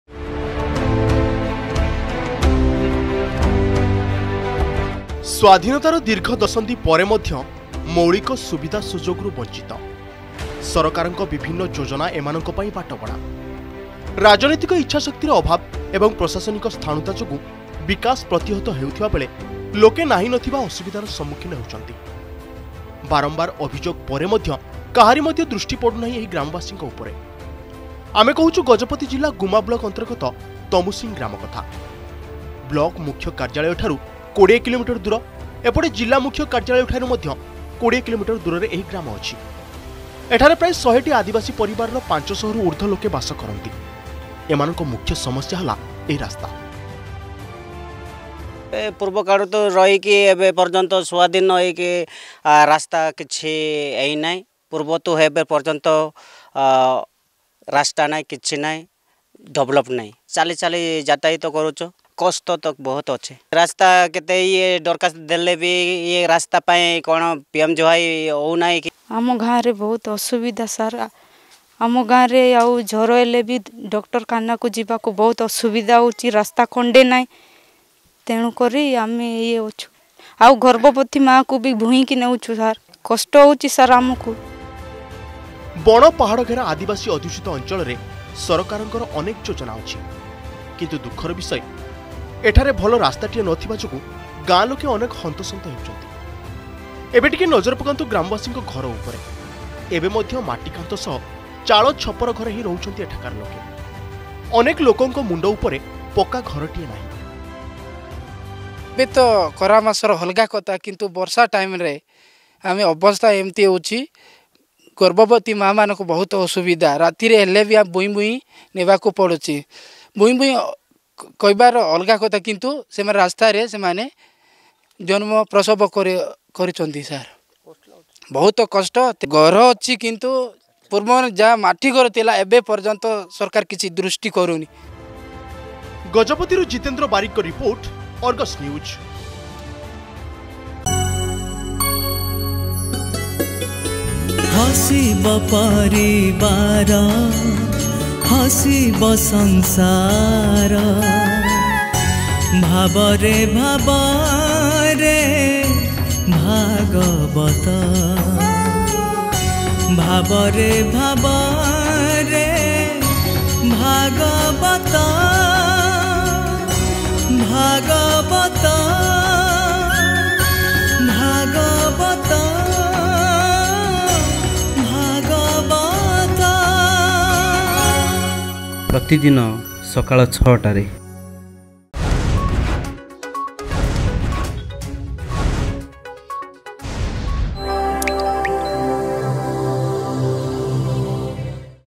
ସ୍ଵାଧୀନତାର ଦୀର୍ଘ ଦଶନ୍ଧି ପରେ ମଧ୍ୟ ମୌଳିକ ସୁବିଧା ସୁଯୋଗରୁ ବଞ୍ଚିତ !
ସରକାରଙ୍କ ବିଭିନ୍ନ ଯୋଜନା ଏମାନଙ୍କ ପାଇଁ ବାଟବଣା । ରାଜନୈତିକ ଇଛାଶକ୍ତିର ଅଭାବ ଓ ପ୍ରଶାସନିକ ସ୍ଥାଣୁତା ଯୋଗୁଁ ବିକାଶ ପ୍ରତିହତ ହେଉଥିବା ବେଳେ ଲୋକେ ନାହିଁ ନଥିବା ଅସୁବିଧାର ସମ୍ମୁଖିନ ହେଉଛନ୍ତି।  ଆସନ୍ତା ଦେଖିବା ଗଜପତି ଜିଲ୍ଲାରୁ ଏହି ରିପୋର୍ଟ